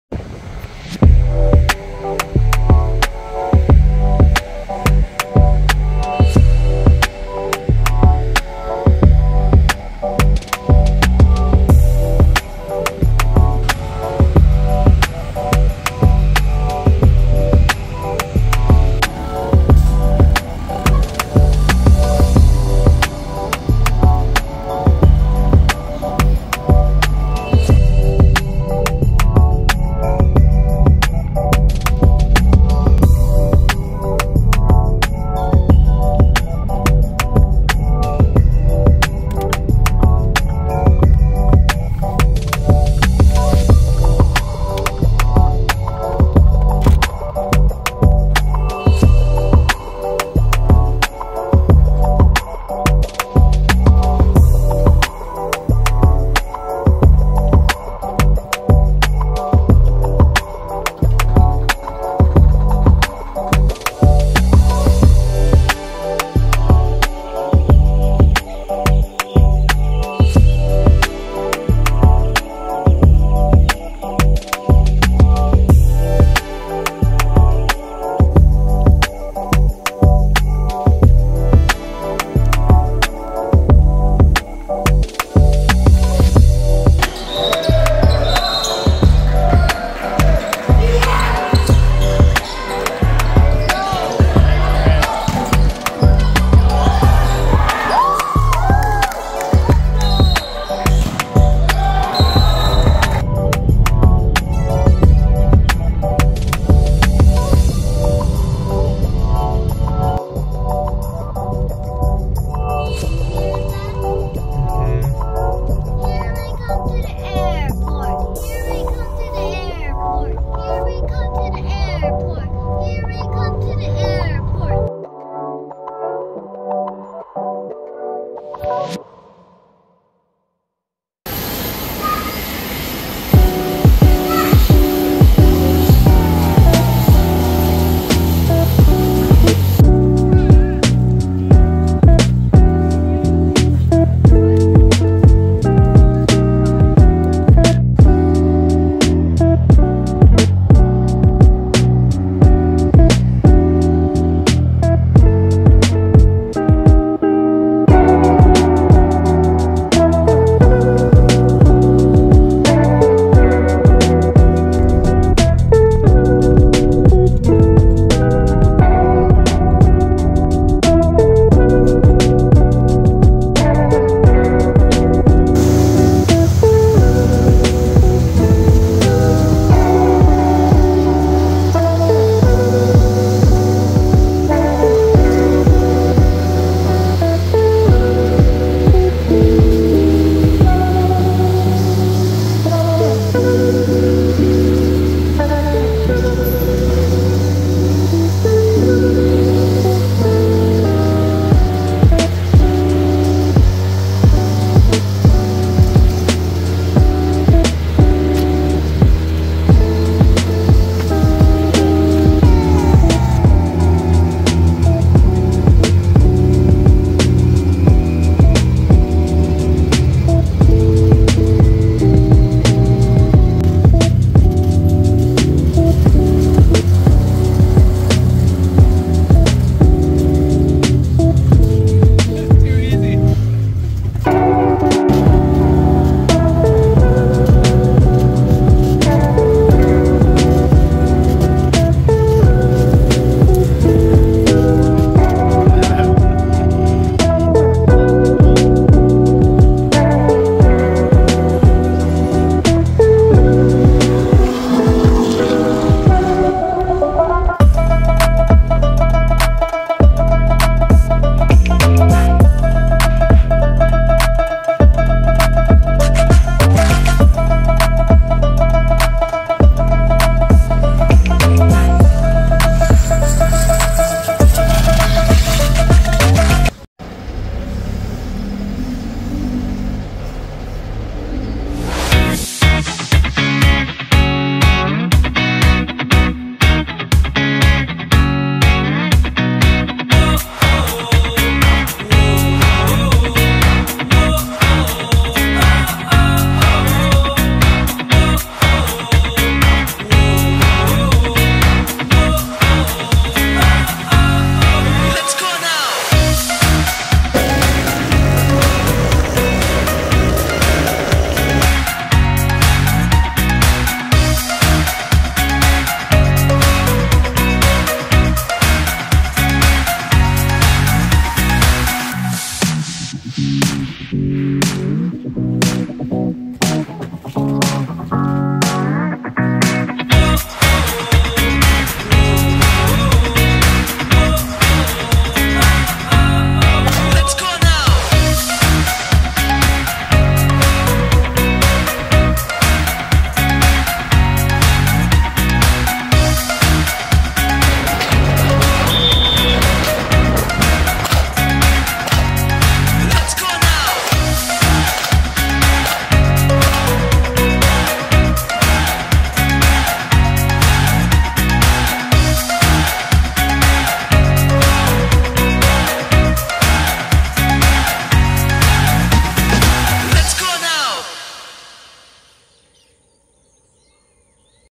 Random Memorable Moments In Belize
Decided to document different things in my life that I found memorable. These are from Jan 2024 till April 2024.